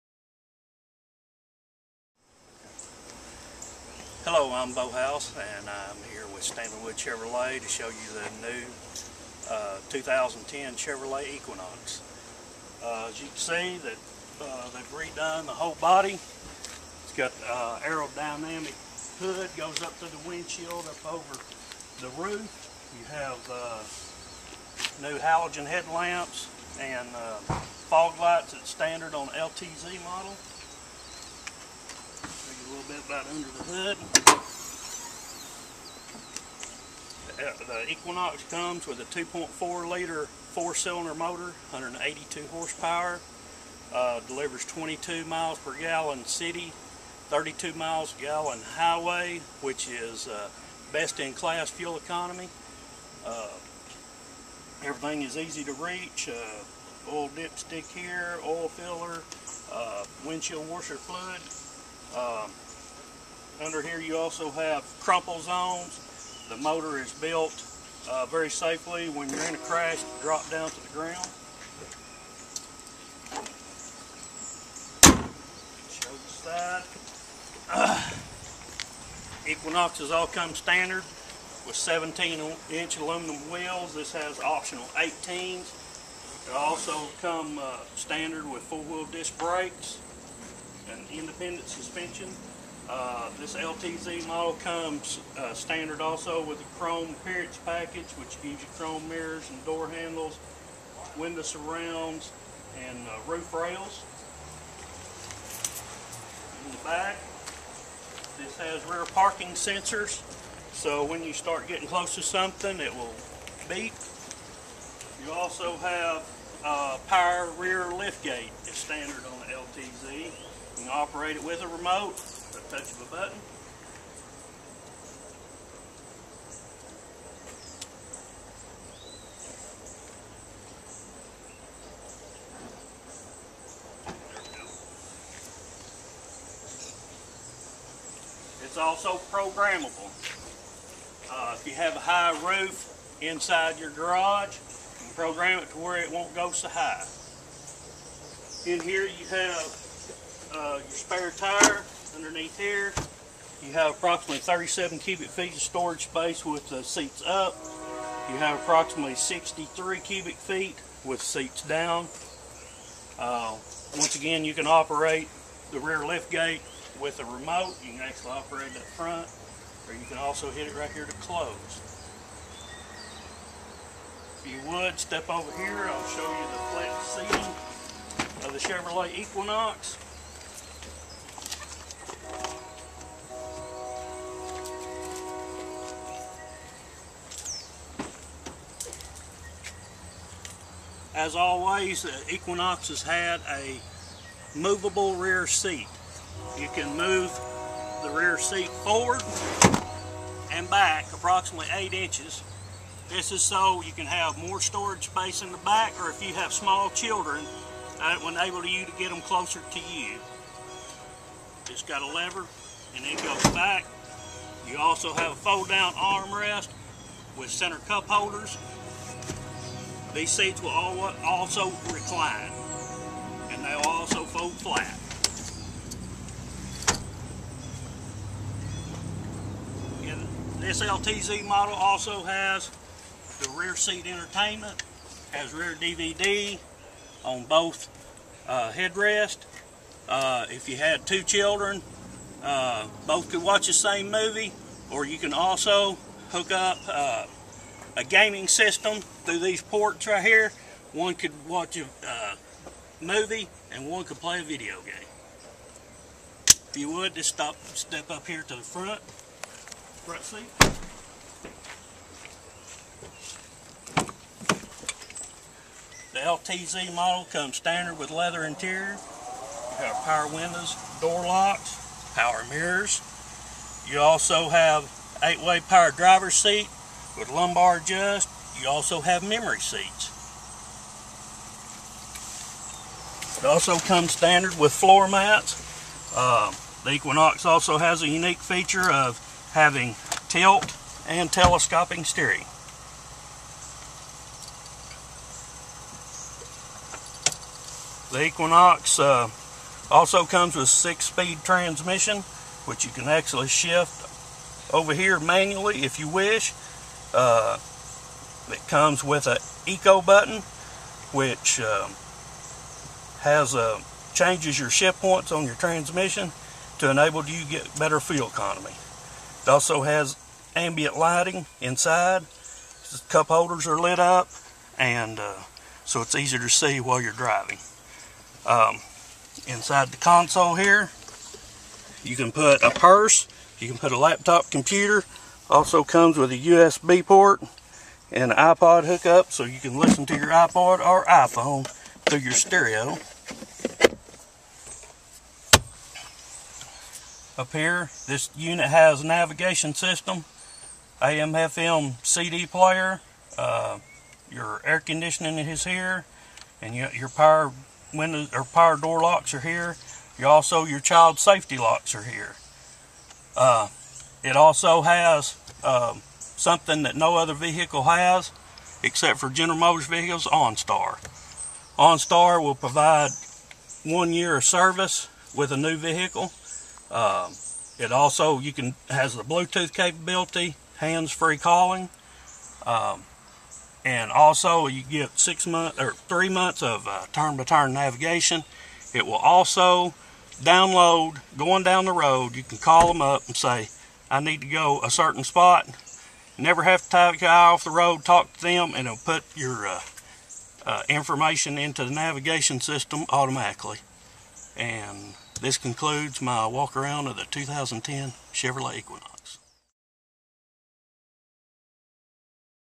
2010 Chevy Equinox Stanley Wood Chevrolet Walk Around
Bo is one of Stanley Wood Chevrolet's knowledgeable salesmen.  Come let him show the Chevy Equinox and all of our great Chevrolet vehicles we have to offer.